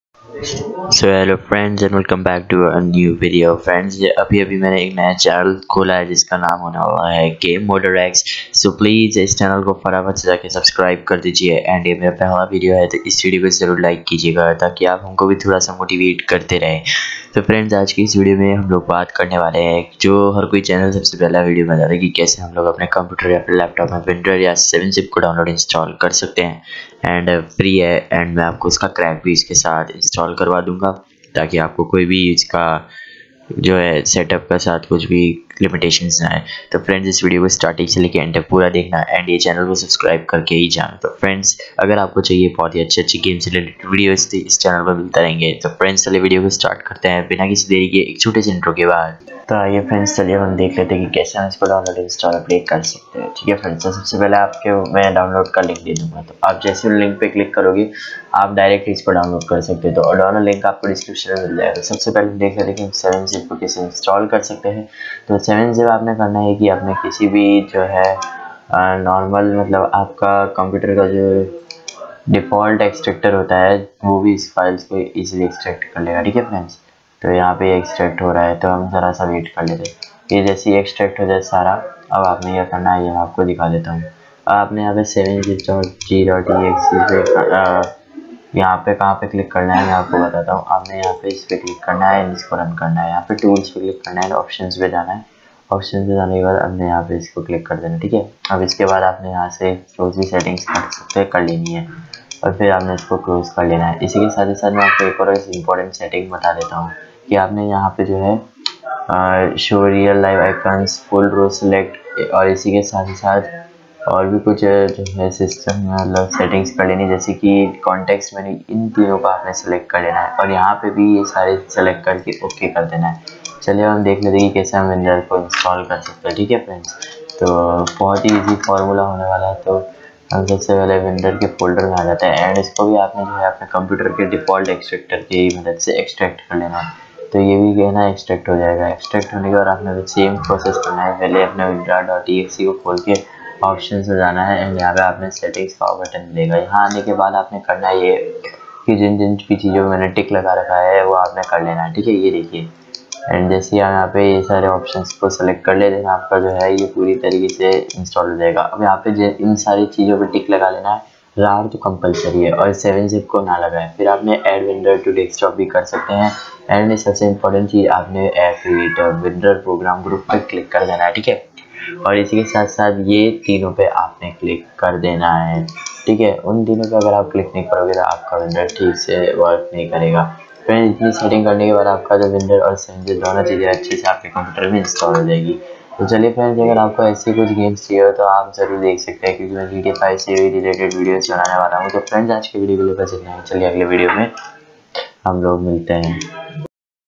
How to download WinRar \7Zip for PC
Hello guys and welcome back to my first ever video on how to download / install Winrar / 7Zip , ? GB RAM / ? Processor , 32 bit/ 64 bit Windows 10/7/8/8.1 .

Comment if you have any problems

🌹IMPORTANT🌹
DO NOT EXTRACT JUST INSTALL THE APPLICATION
🌹IMPORTANT🌹

🌹Download WinRar🌹

🌹Download 7Zip🌹
🔑32-bit x86🔑
🔑32-bit x86🔑

🔑64-bit x64🔑
🌹Download 7Zip🌹

🌹For More Vidos🌹